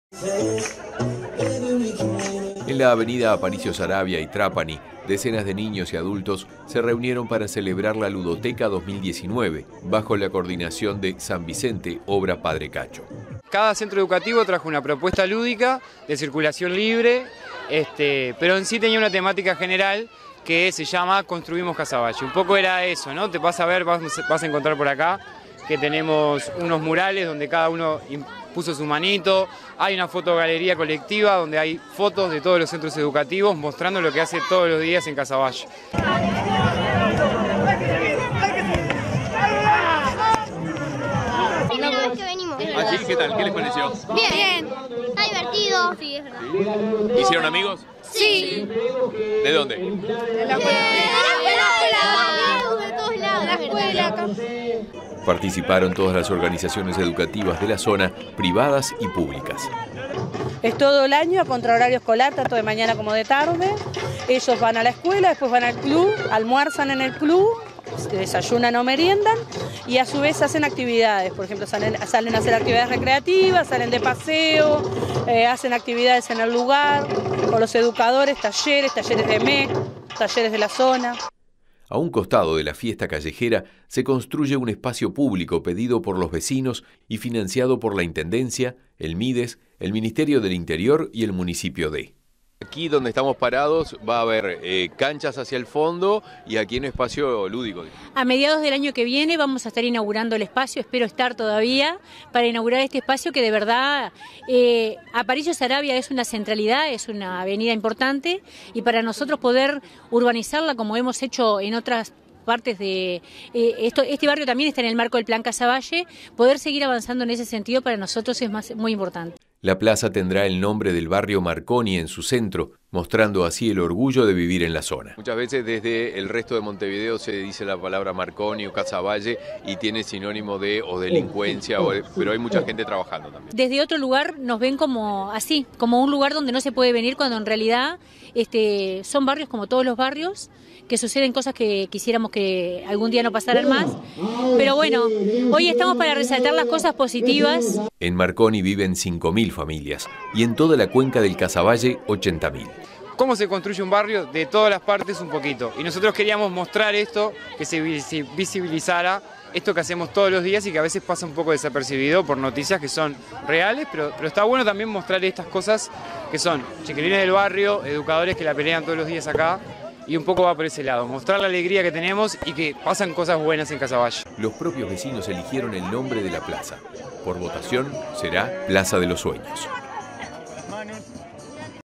PLAZA MARCONI TNU Rafael Demetrio Guardia
Montevideo Municipio D Barrio Marconi Casavalle juegos ludicos callejeros eligen nombre de la plaza de los sueños. Obra Padre Cacho. Diego Sotelo.